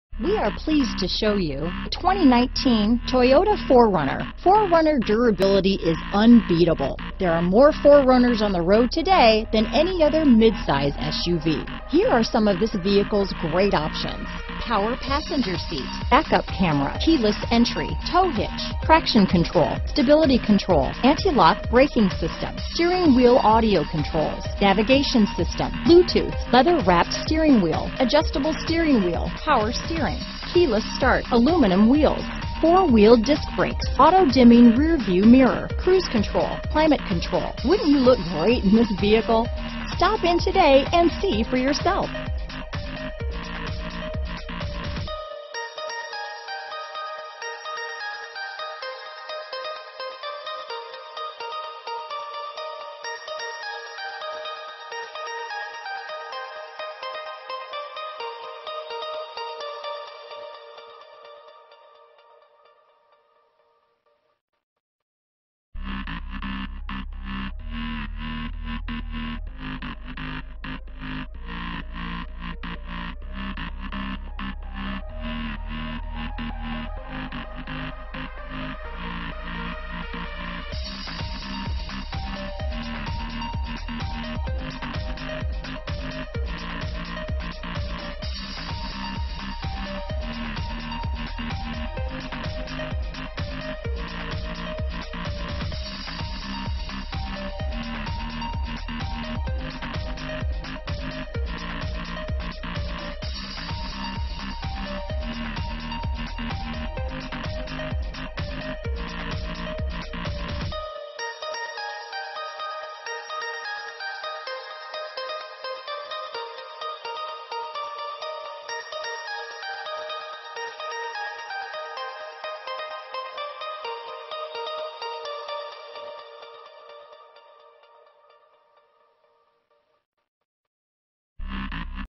2019 Toyota 4Runner Claremont, Ontario, Pasadena, Anaheim, CA 00193827
New 2019 Toyota 4Runner available in Claremont, California at Claremont Toyota. Servicing the Ontario, Pasadena, Anaheim, CA area.

Claremont Toyota
601 Auto Center Drive